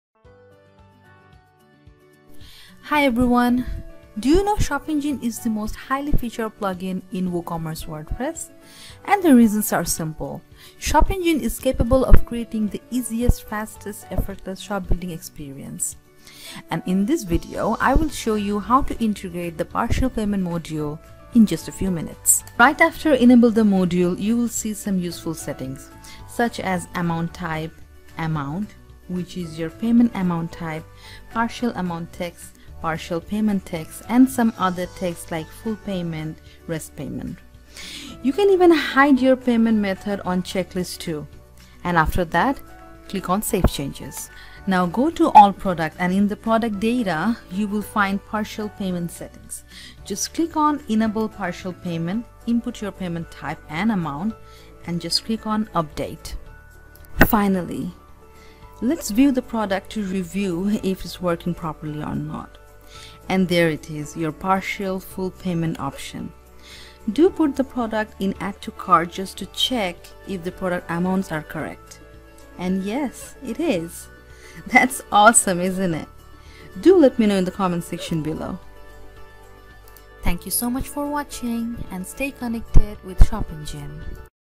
How to Activate Partial Payment Option | ShopEngine
Increase conversion rate by adding a partial payment facility and help your buyers to get their desired product right away! This module can provide your consumers with a great experience and flexibility if they can't pay the whole amount up front while also helping you prevent losing orders.

TIMESTAMPS
0:00 Intro
0:21 How to Activate and Configure Partial Payment Module
0:43 How to Enable Partial Payment on Products
1:19 Endnote